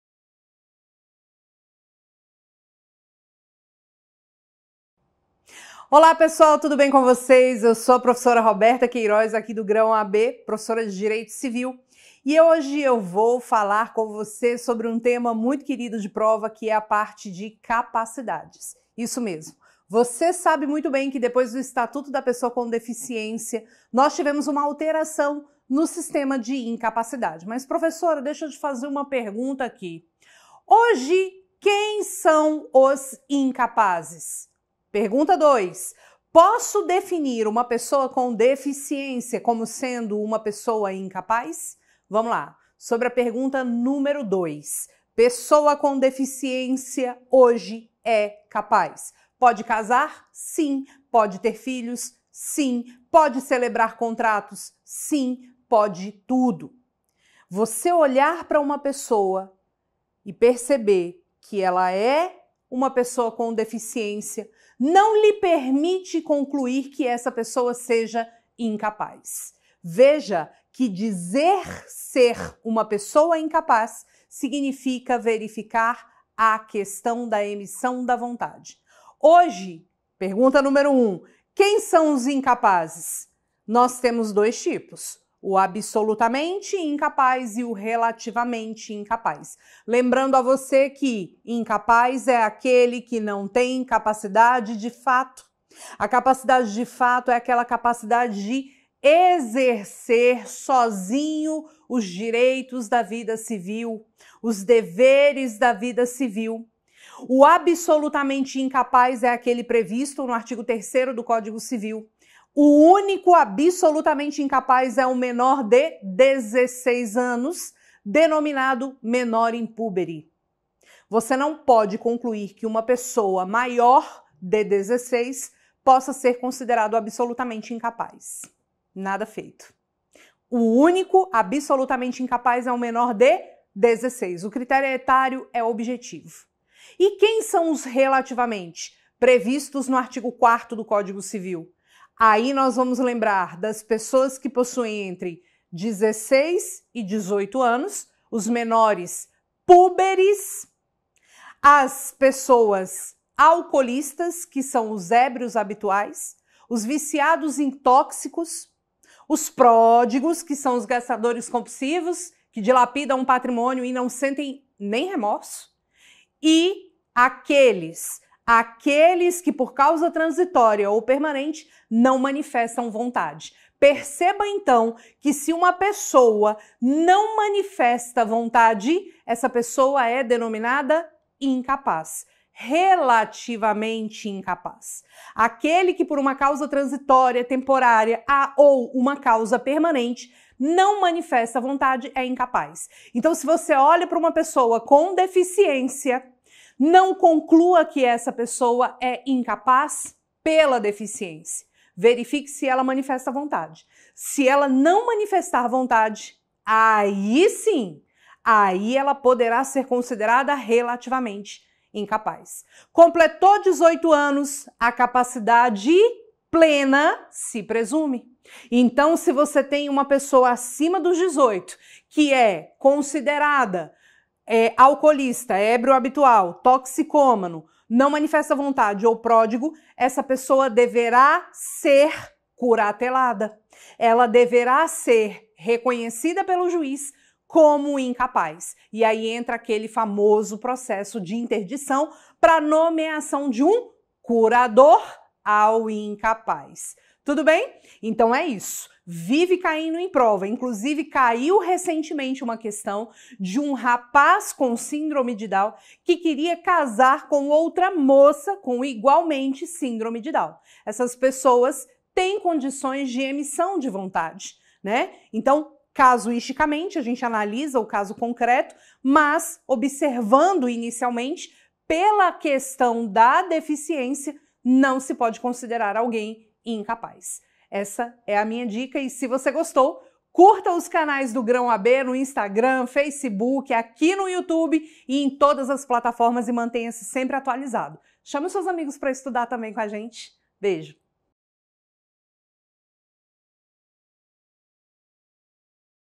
OAB Express: Dica sobre Capacidade para a 1ª fase do Exame da OAB!
Neste vídeo, a professora Roberta Queiroz trará dicas pra você entender e acertar questões de Capacidade na 1ª fase do Exame da OAB.